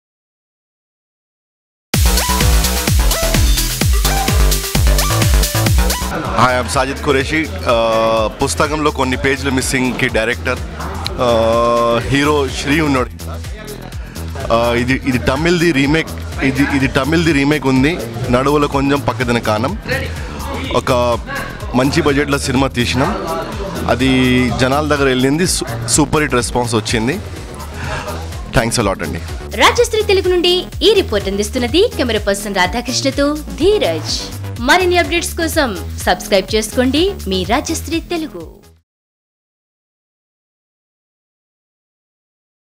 Excluisve Interview : Pustakamlo Konni Pagelu Missing Director Sajid Qureshi [HD]
RajshriTelugu Excluisve Interview : Pustakamlo Konni Pagelu Missing Director Sajid Qureshi [HD]. Check out the exclusive interview of Sajid Qureshi, the director of Sri's Pustakamlo Konni Pagelu Missing, in this HD 1080p video.